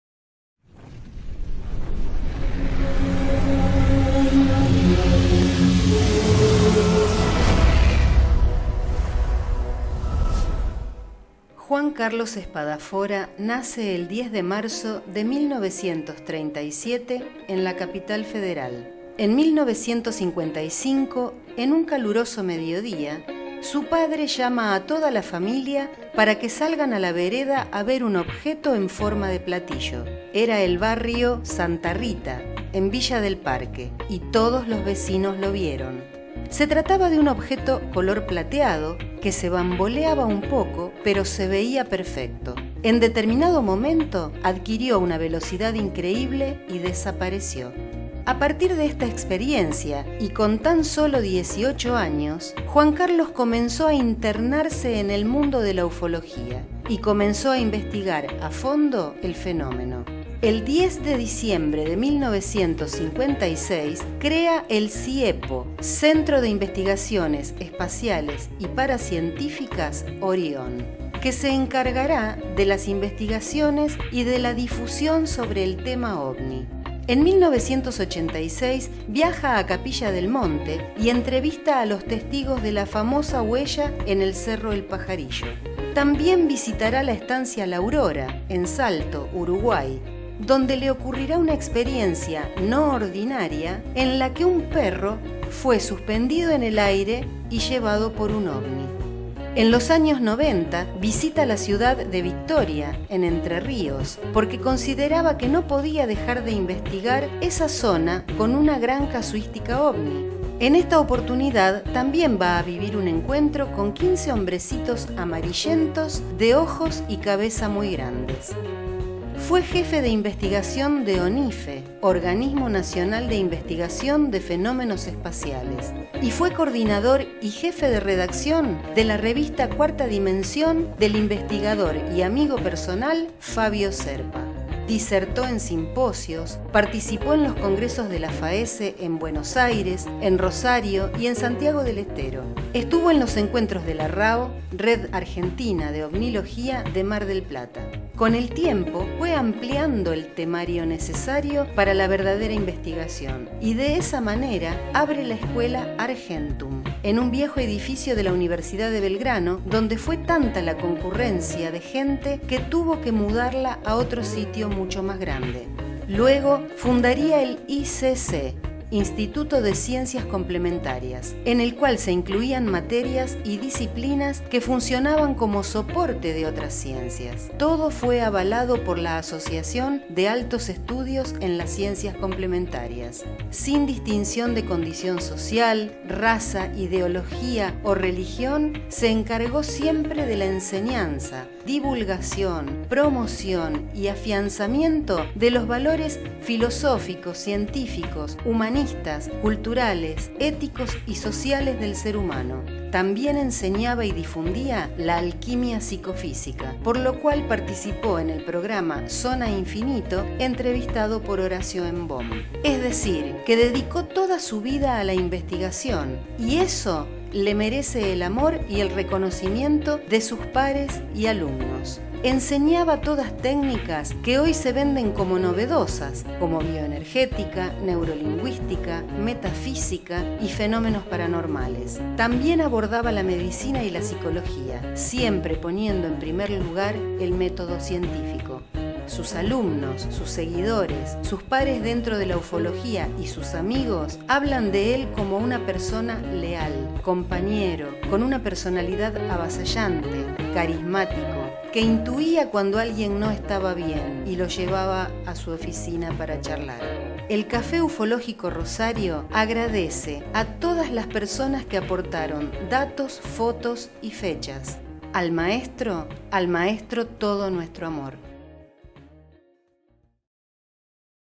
INFORME SOBRE JUAN CARLOS SPADAFORA
Juan Carlos fue profesor y maestro de muchos investigadores dentro de la Ufología pero por sobre todas las cosas, un amigo.
Café Ufológico Rosario, quiere hacer un reconocimiento más que merecido.
Informe introductorio de programa en vivo realizado en el Canal Demonios.